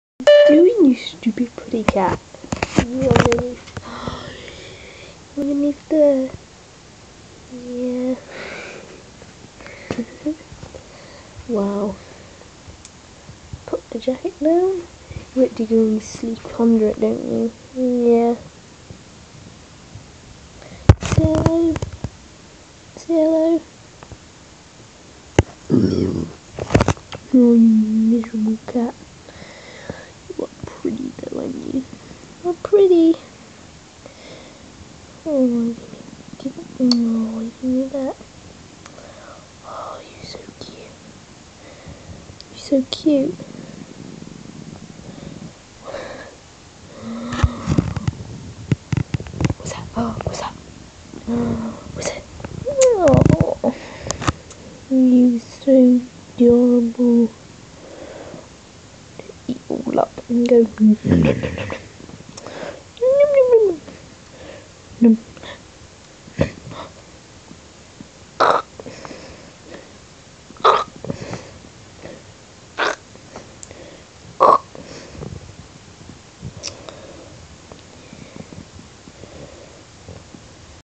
My adorable cat
My cat fell asleep under my jacket